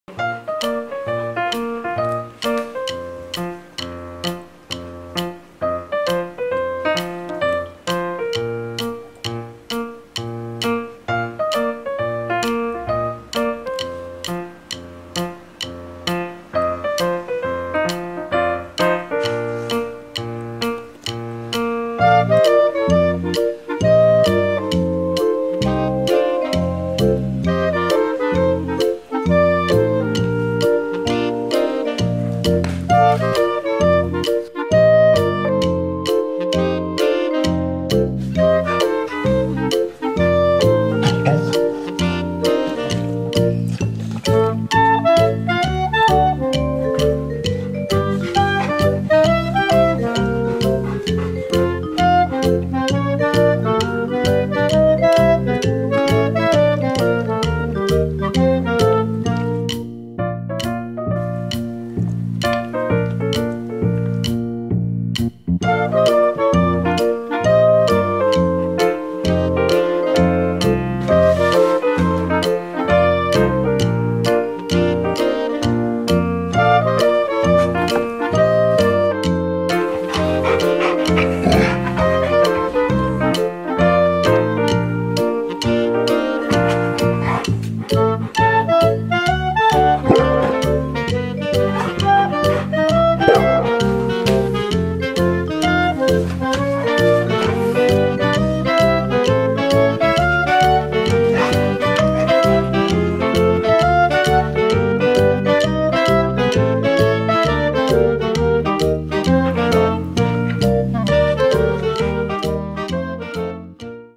What Does My Dog Do When the Rabbit Ignores Him
Guys, look what Bailey's Golden Retriever does when Sam Bunny ignores him! Like a dog trying to play with a rabbit. 🔔and leave your comments under this video!